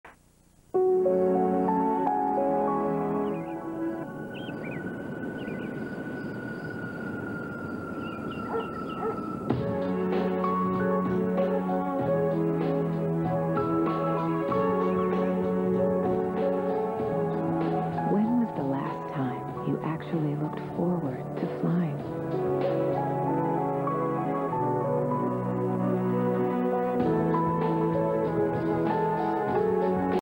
2001 Asiana Airlines Commercial
A child swings from an old tree while an Asiana 747 flys overhead in this 2001 commercial.